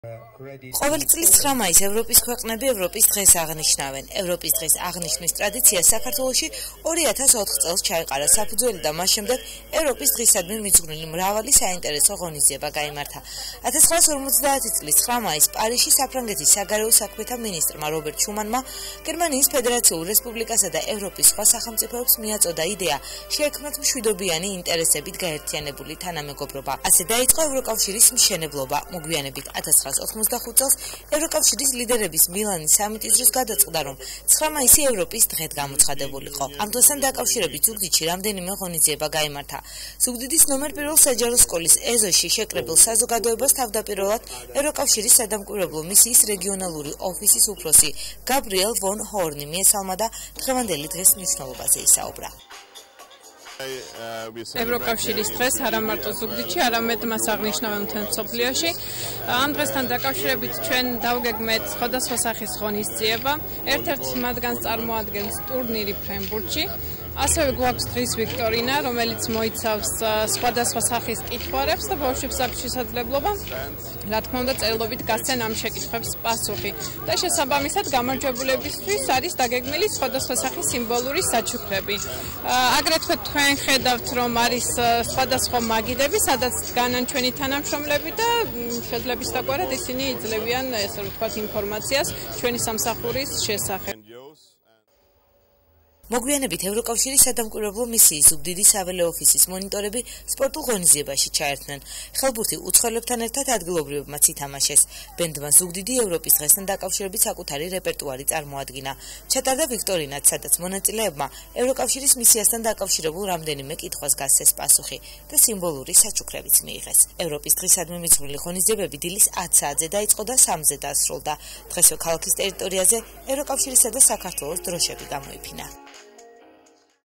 9 მაისს ევროპის ქვეყნები ევროპის დღეს აღნიშნავენ.
ყოველი წლის 9 მაისს ევროპის ქვეყნები ევროპის დღეს აღნიშნავენ.  ევროპის დღის აღნიშვნის ტრადიციას საქართველოში 2004 წელს ჩაეყარა საფუძველი  და მას შემდეგ ევროპის დღისადმი მიძღვნილი მრავალი საინტერესო ღონისძიება გაიმართა.

1950 წლის 9 მაისს პარიზში საფრანგეთის საგარეო საქმეთა მინისტრმა რობერტ შუმანმა გერმანიის ფედერაციულ რესპუბლიკასა და ევროპის სხვა სახელმწიფოებს მიაწოდა იდეა, შეექმნათ მშვიდობიანი ინტერესებით გაერთიანებული თანამეგობრობა.ასე დაიწყო ევროკავშირის მშენებლობა.მოგვიანებით, 1985 წელს, ევროკავშირის ლიდერების მილანის სამიტის დროს გადაწყდა, რომ 9 მაისი ევროპის დღედ გამოცხადებულიყო.

ამ დღესტან დაკავშირებით ზუგდიდში რამდენიმე ღონისძიება გაიმართა, ზუგდიდის N1 საჯარო სკოლის ეზოში შეკრებილ საზოგადოებას თავდაპირველად ევროკავშირის სადამკვირვებლო მისიის რეგიონალური ოფისის უფროსი გაბრიელ ვონ ჰორნი მიესამა და დღევანდელი დღის მნიშვნელობაზე ისაუბრა.მოგვიანებით ევროკავშირის სადამკვირვებლო მისიის ზუგდიდის საველე ოფისის მონიტორები სპორტულ ღონისძიებაში ჩერთვნენ, ხელბურთი უცხოელებთან ერთად ადგილობრივებმაც ითამაშეს.ბენდმა „ზუგდიდი" ევროპის დღესთან დაკავშირებით საკუთრი რეპერტუარი წარმოადგინა.ჩატრდა ვიქტორინაც,სადაც მონაწილეებმა ევროკავშირის მისიათან დაკავშირებულ რამდენიმე კითხვას გასცეს პასუხი და სიმბოლური საჩუქრებიც მიიღეს.ევროპის დღისადმი მიძღვნილი ღონისძიებები დილის 10 საათზე დაიცყო და 3--ზე დასრულდა. დღესვე ქალაქის ტერიტორიაზე ევროკავშირისა და საქართელოს დროშები გამოიფინა.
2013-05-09